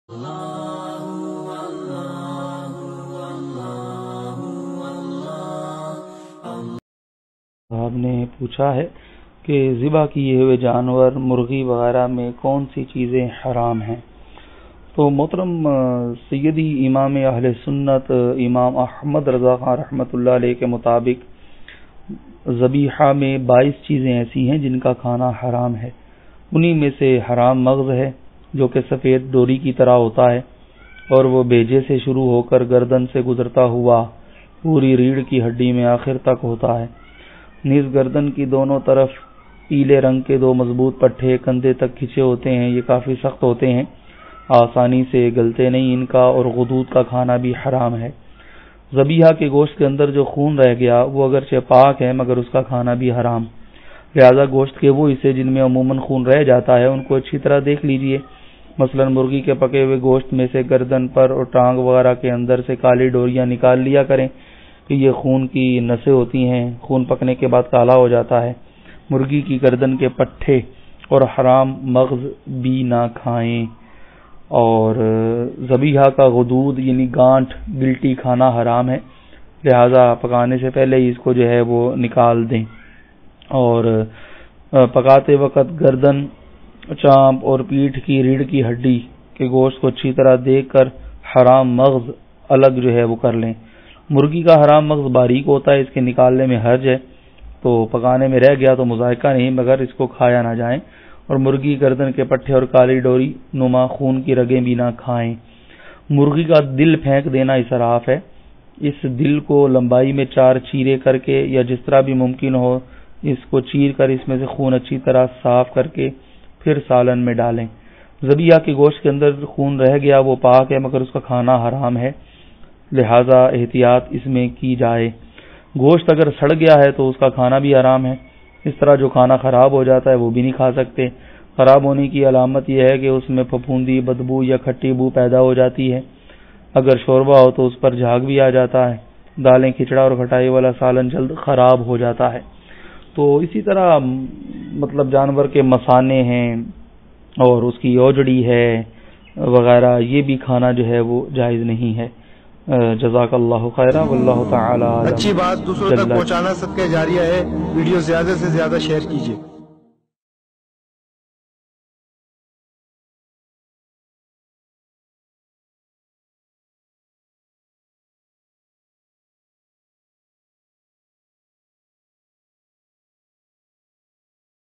Zaba (Slaughter) Janwar ke Kon Konse Cheez Haram hai?
Is Video Me Jannye Ke JAnwar me kon kose Cheeze Kha Sakte hai AUr Konse Nahi?
Videos ko Like kare aur apne doosto ke sath zarur share kare.
facebook page be like kaare..